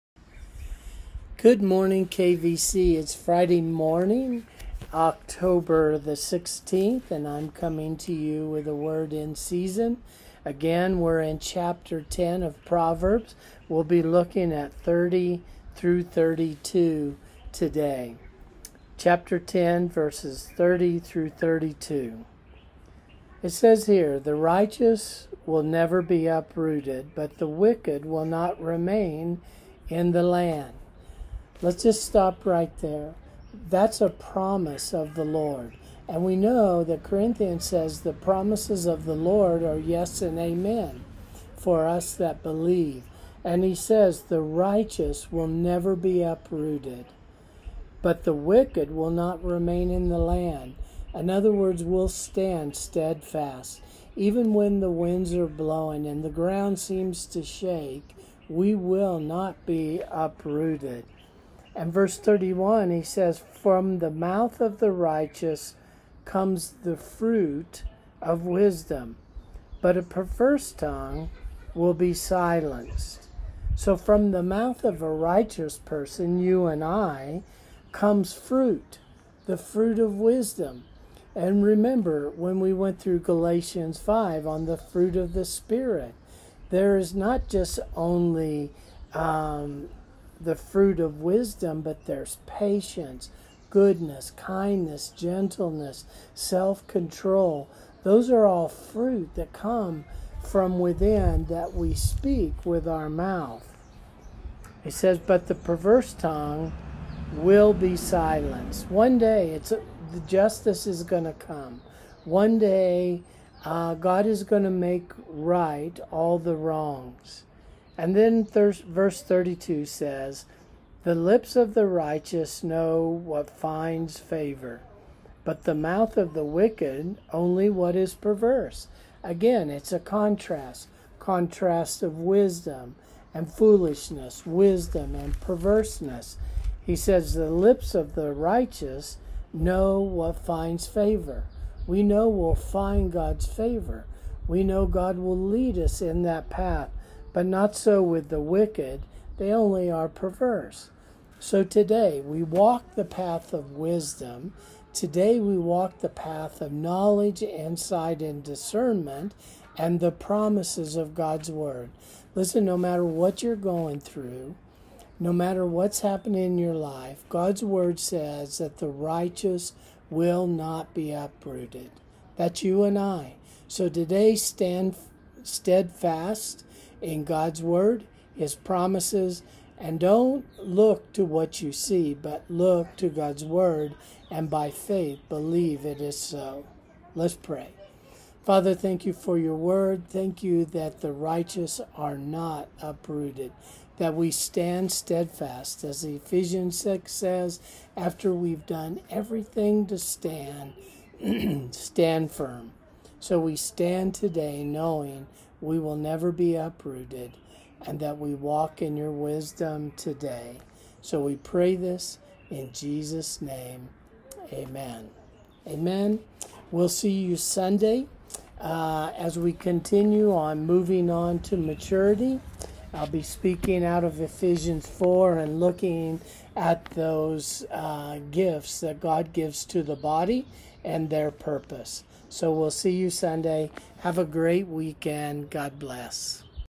THE RIGHTEOUS WILL NEVER BE UPROOTED // WORD IN SEASON // OCTOBER, 16TH 2020
Pastor Craig shares from Proverbs 10:30-32. Walk the path of wisdom no matter what you are facing in life. Stand on God’s Word and His promises.